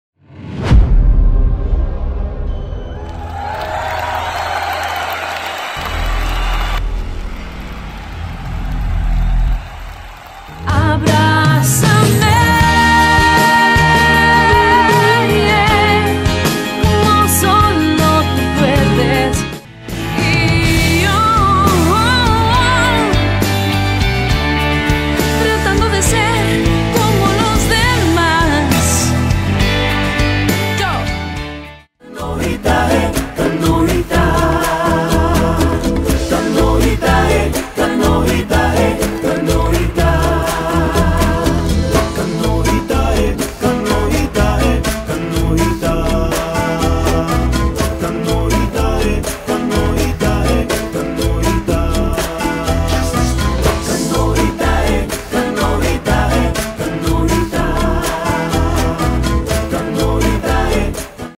Trailer Concierto Sinfónico Paulina Aguirre
Un gran evento en la ciudad de Quito se disfruto junto a la Orquesta sinfónica del Ecuador , y la ganadora del Grammy Ecuatoriana Paulina Aguirre. Este evento grabamos todo el concierto tanto audio como Video un gran reto pero se lo logró
Dirección Audiovisual y Edición: Sebas Sandoval
Ing en Sonido: Gero Silveti
Dirección Musical: Pablo Aguirre

Mucho fruto